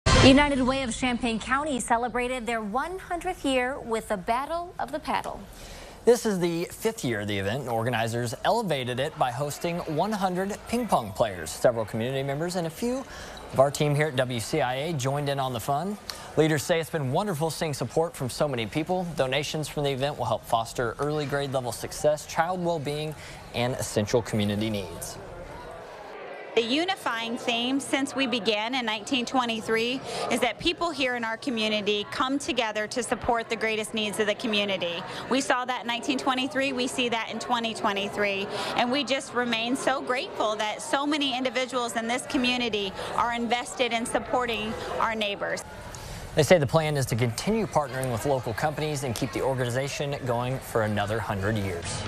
United Way of Champaign hosts annual Battle of the Paddle
United Way of Champaign County kicked off its 100th year with its annual Battle of the Paddle on Wednesday.